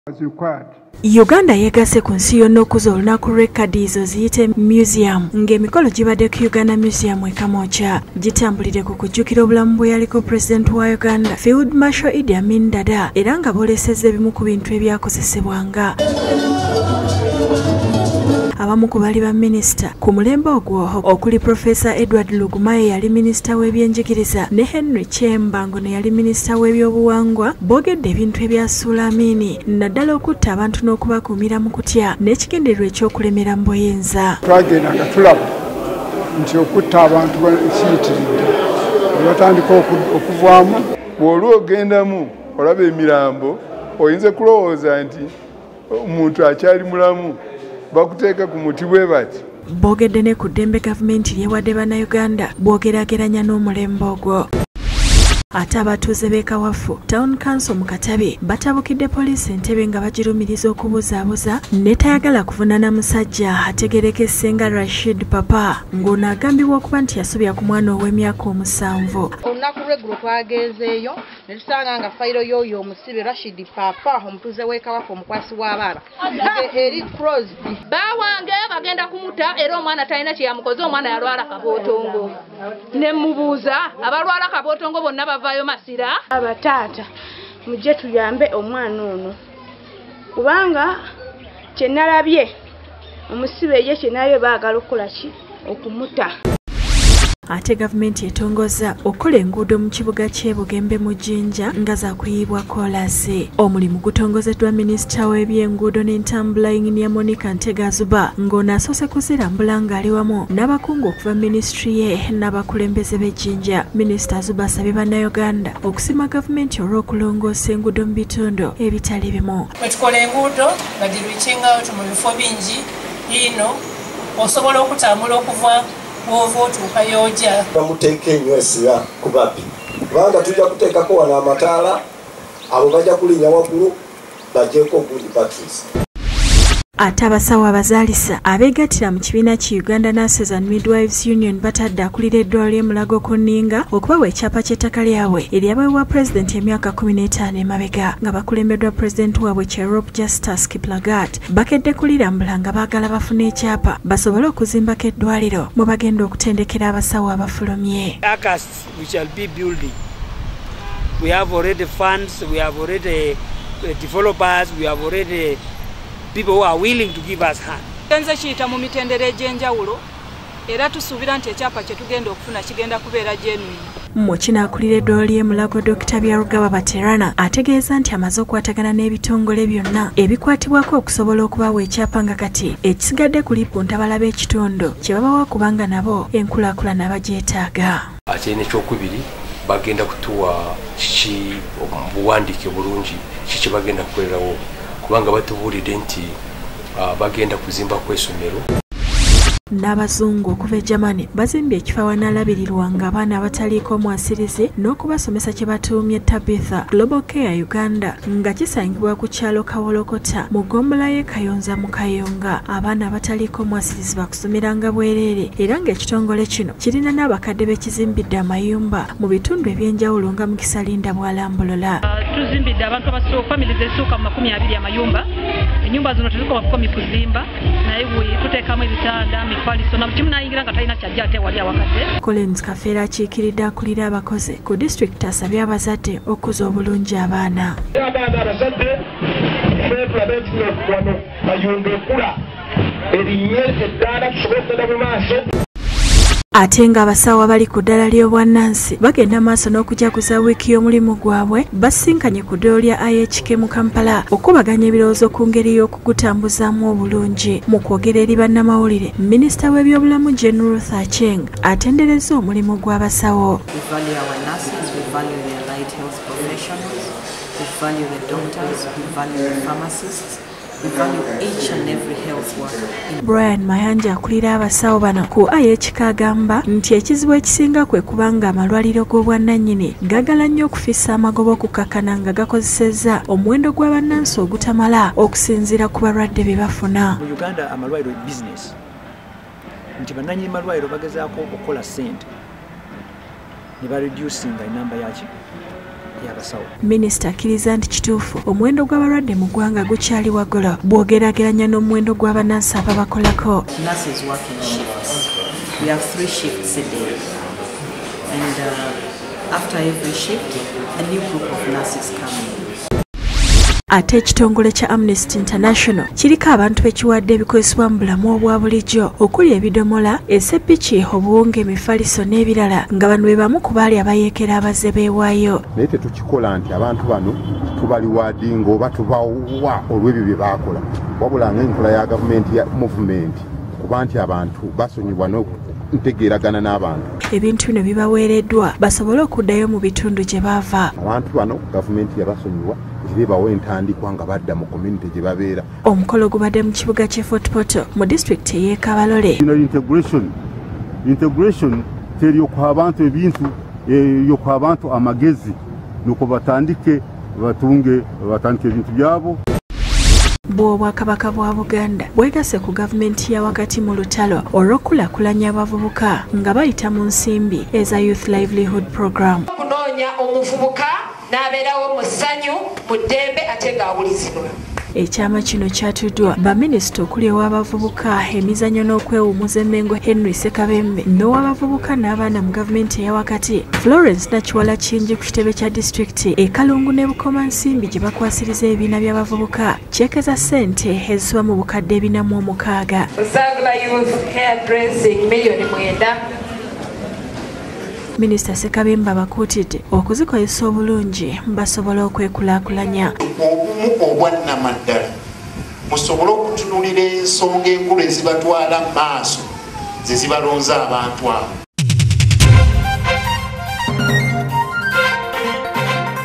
BRIEFS: Okukuza olunaku lwa Museum.
- Abatuuze batabukude Poliisi. Rashid Papa agambibwa okusobya ku mwana ow'emyaka 7. Kyokka Poliisi ebuuzabuuza era yamuyimbudde.
- Gavumenti etongozza okukola enguudo e Jinja.
- Abazungu bazimbye ekifo ky'abaana abatalina mwasirizi n'abakadde.
- Ba Nnansi basisinkanye okwetereeza. Bagamba amalwaliro
ag'obwannanyini gaagala nnyo okufissa amagoba nebakozesa abasawo
abatamala.
- Abaali abayekeera baweereddwa entandikwa.
- Abavubuka e Bukomansimbi bafunye ez'okwekulakulanya. Bakwasiddwa
ceeke za bukadde obusukka mu 406.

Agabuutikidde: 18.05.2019